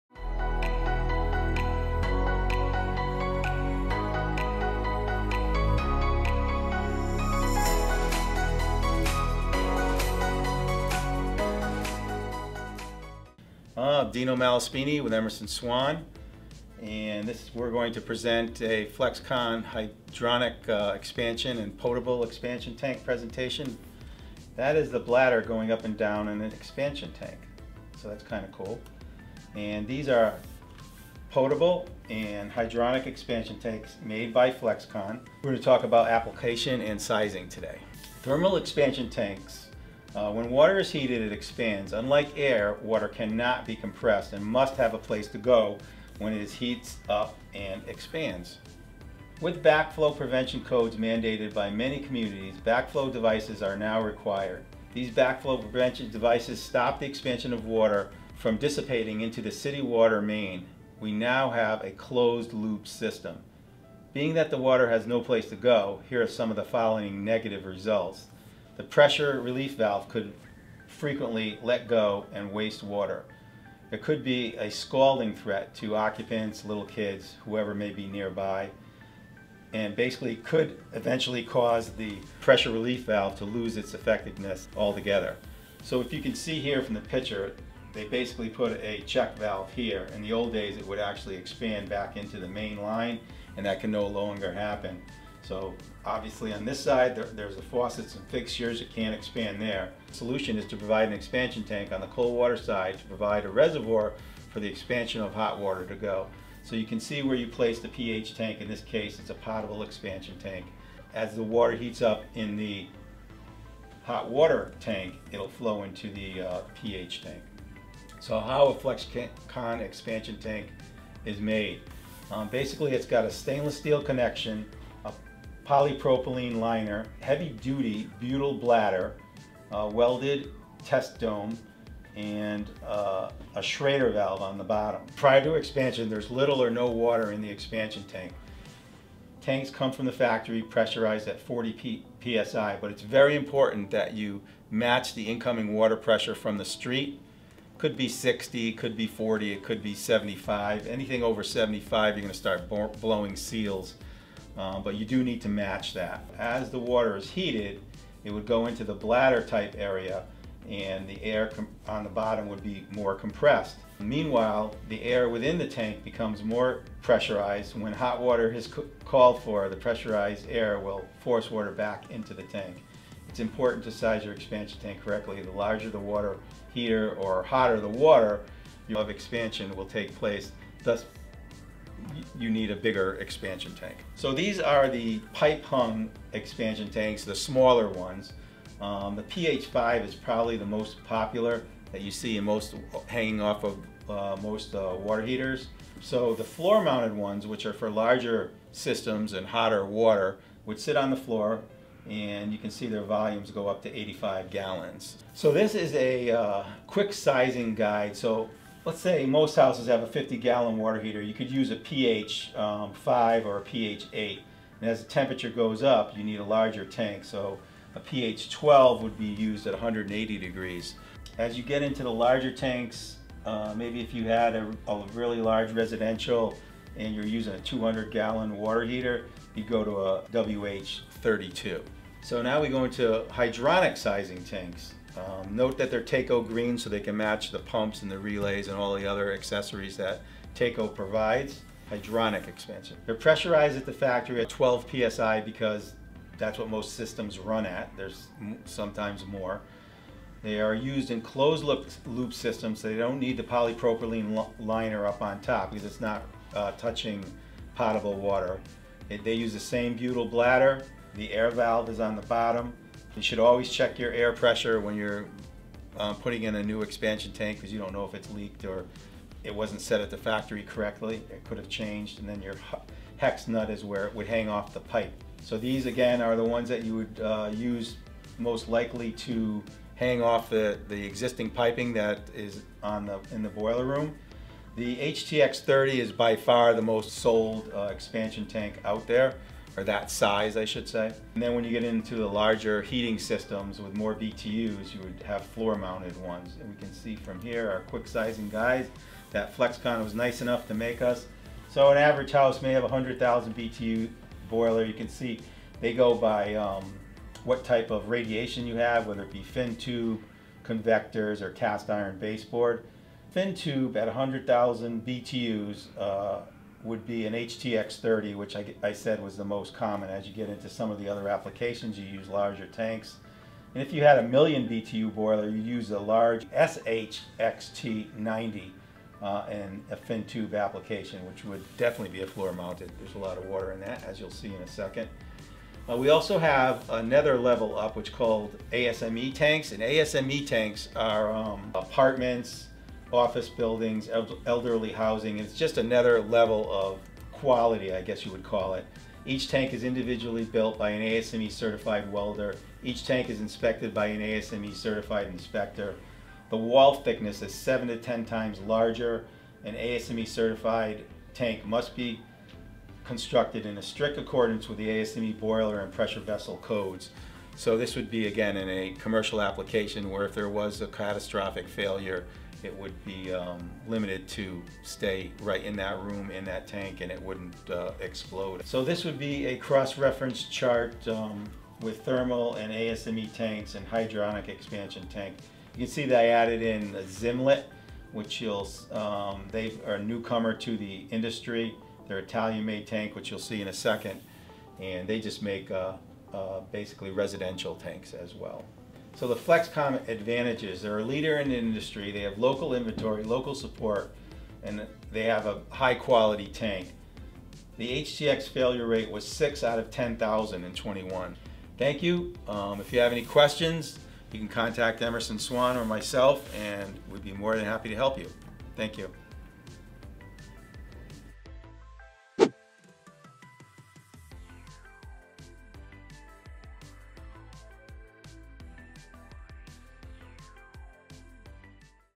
Flexcon - Preasure Tanks FULL OVERVIEW
Dino Malespini of Emerson Swan's Distribution Products Division goes over the full product line of Brasscraft.